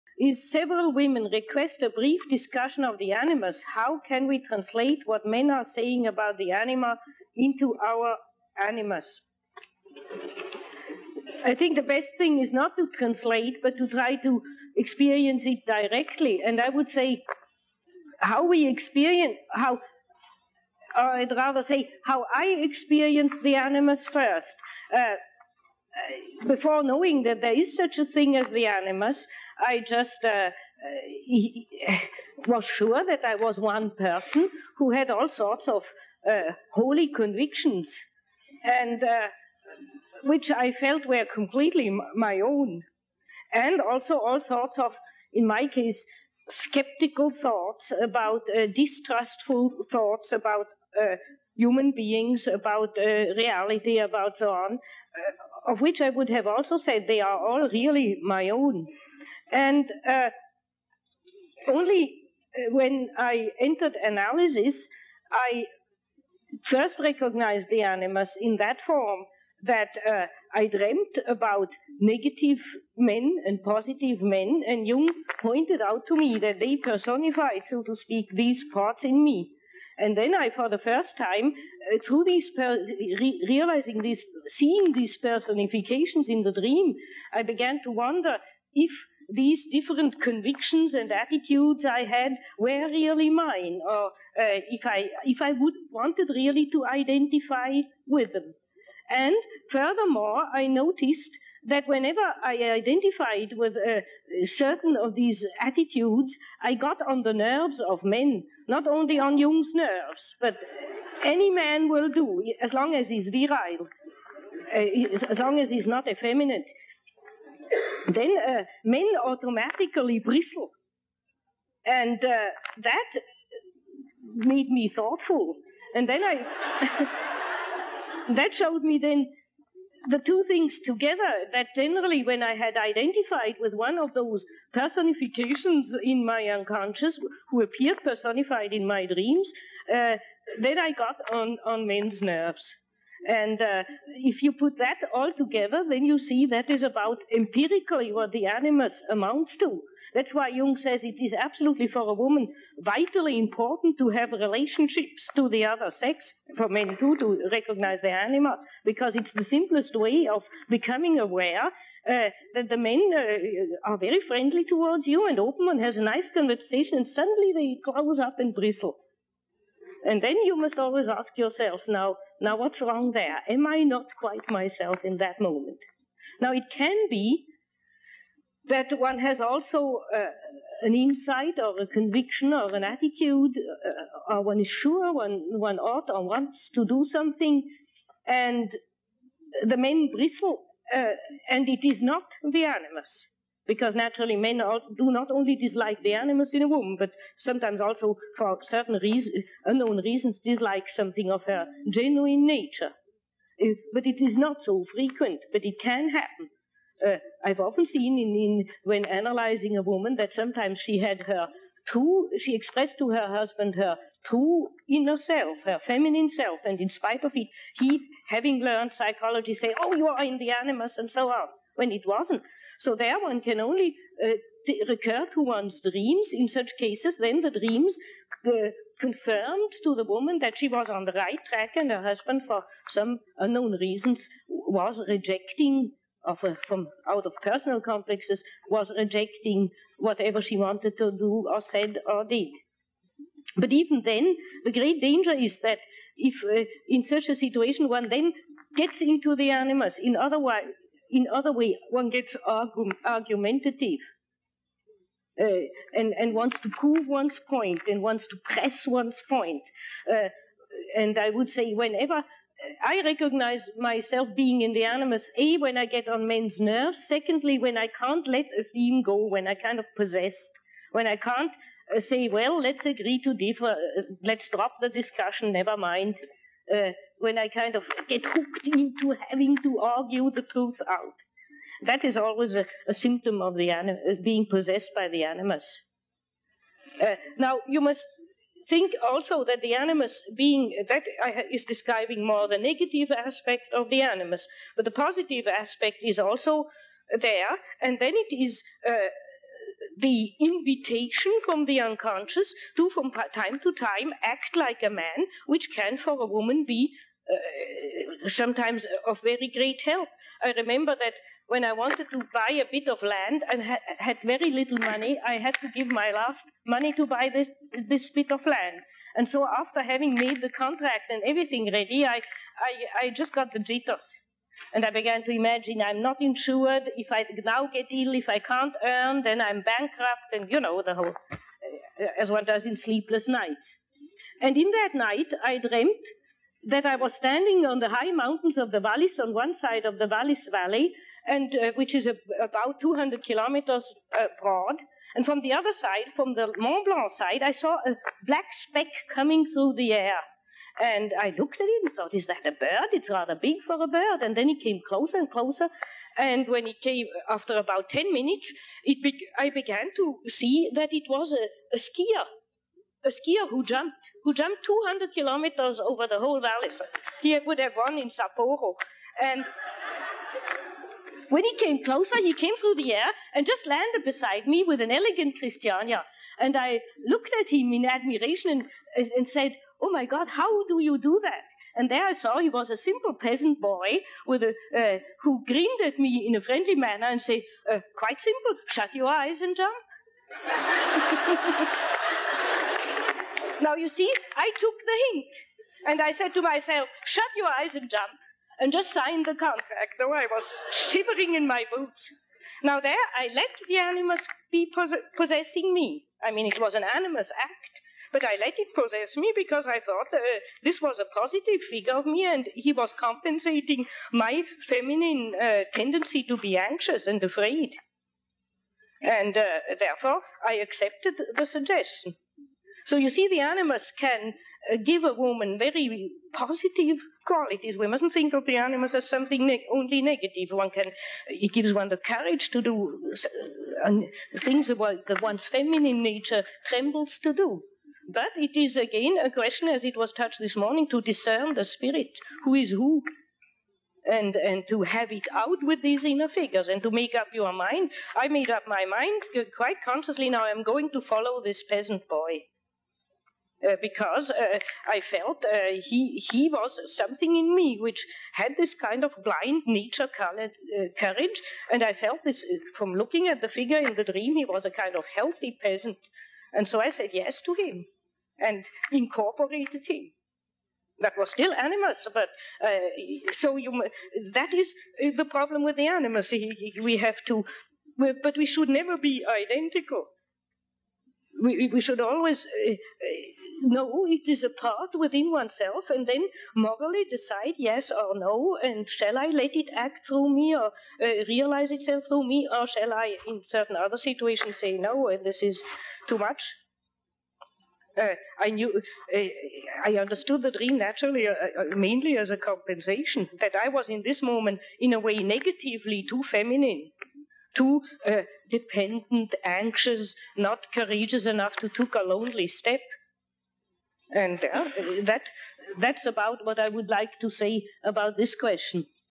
Marie Louise Von Franz - How Women Can Feel Their Animus
Argubly the greatest Jungian analyst after Jung himself, ML von Franz, explains to a group to women how to "find" their animus, and how to know when the animus is active within them.